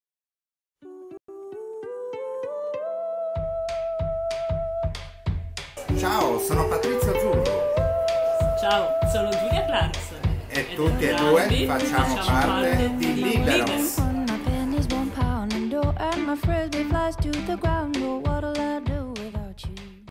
Giulia e Patrizo fanno parte di Lìberos
Giulia Clarkson è una scrittrice e Patrizio Zurru è il suo libraio: entrambi fanno parte di Lìberos, la comunità dei lettori sardi.